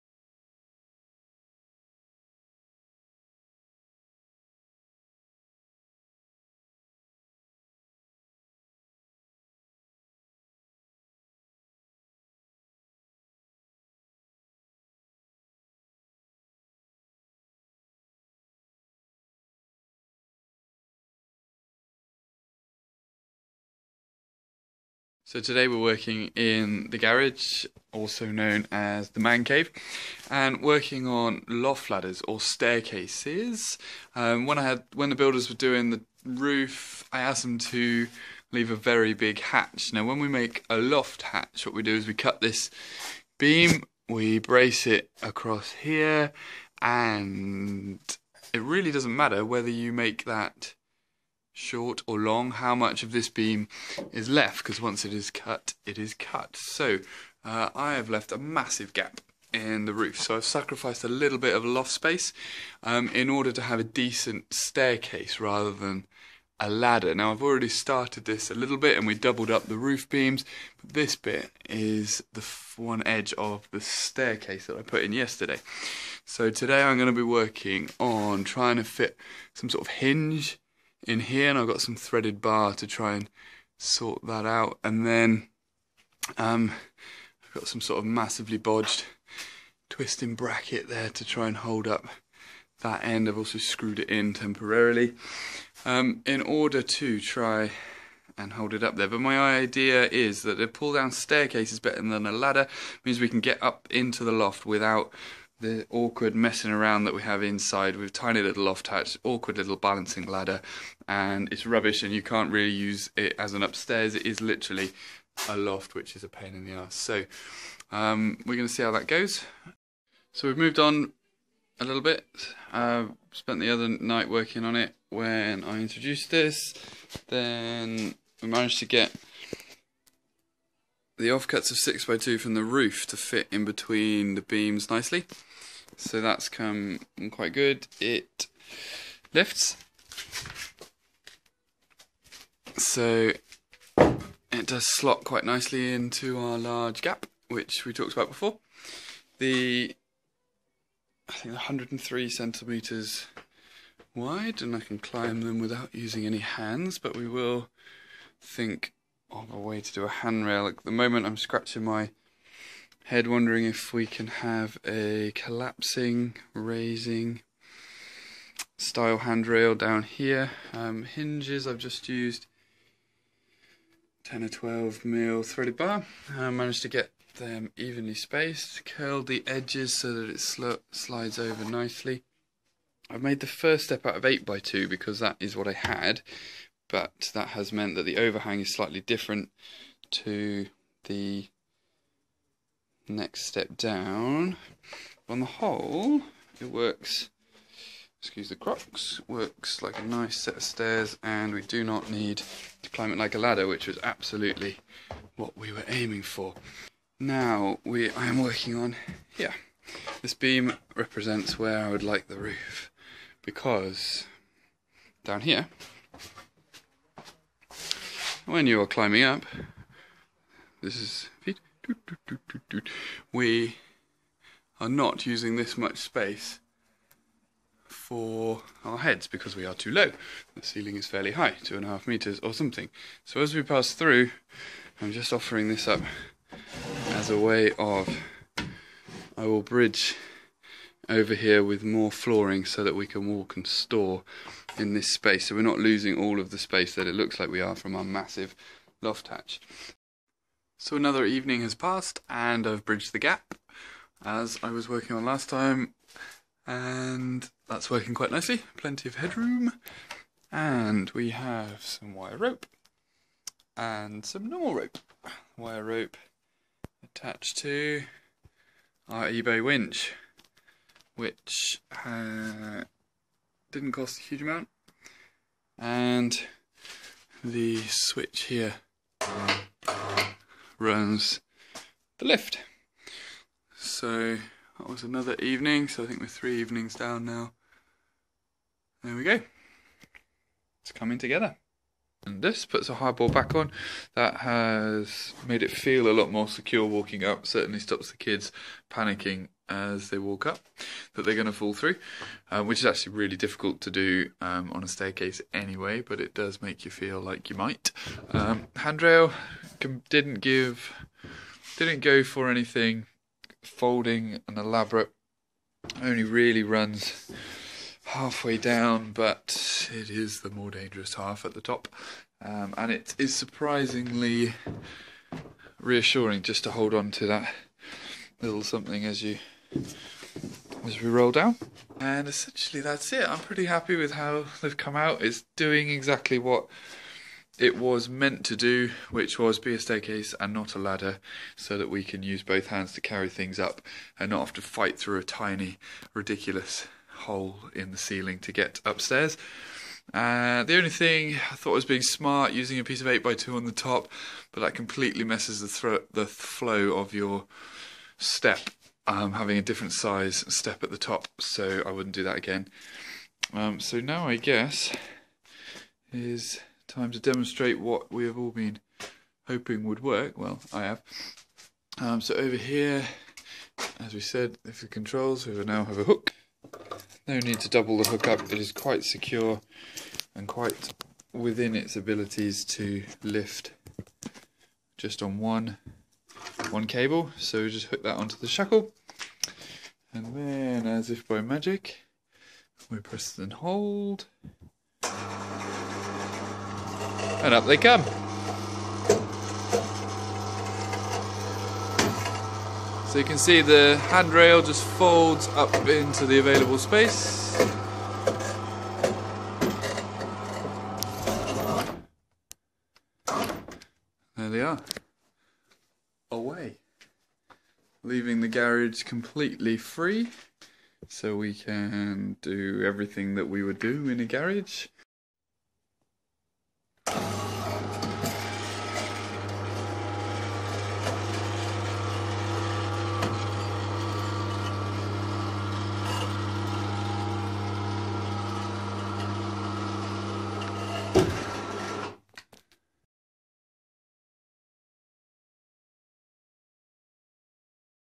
Secret DIY Staircase build
turning loft space into a usable room space using off cuts! without having a massive staircase taking up my workshop!